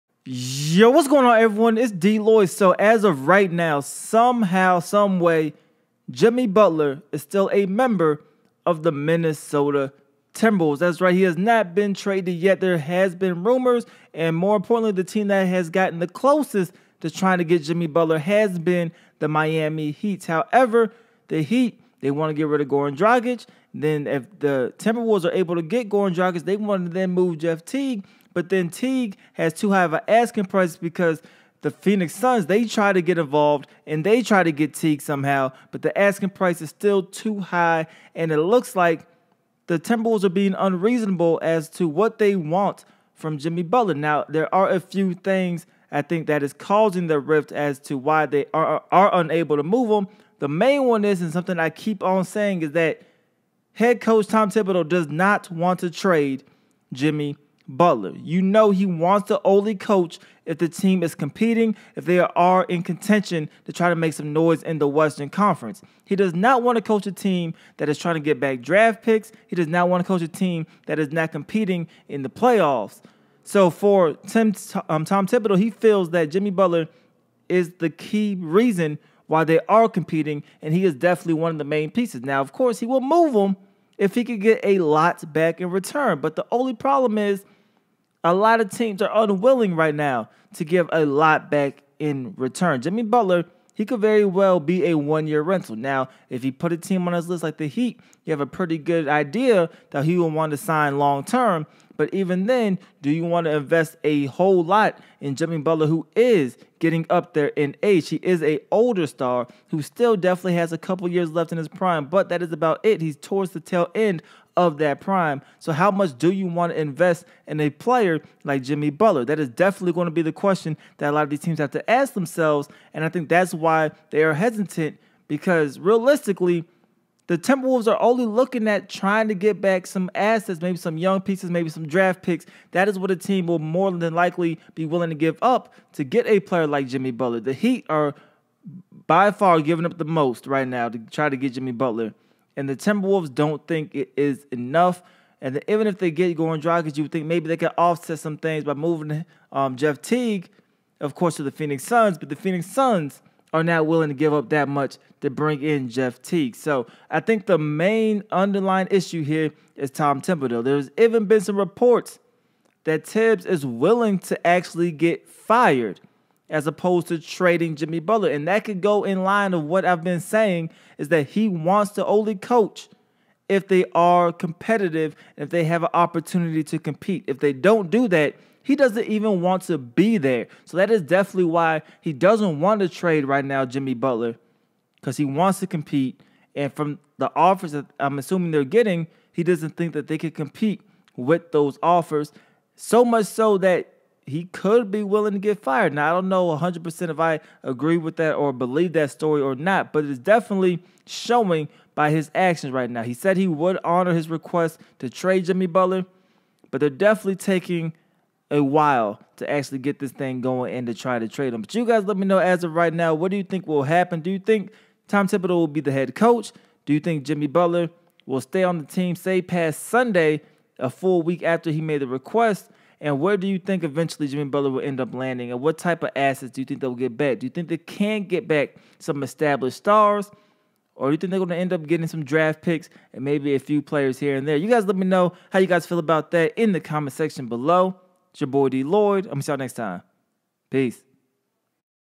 Tom Thibodeau Doesn't Want To Trade Jimmy Butler and Could Prefer To Get Fired Instead!!! | NBA News
Tom Thibodeau Doesn't Want To Trade Jimmy Butler and Could Prefer To Get Fired by Minnesota Timberwolves Instead! | Daily NBA News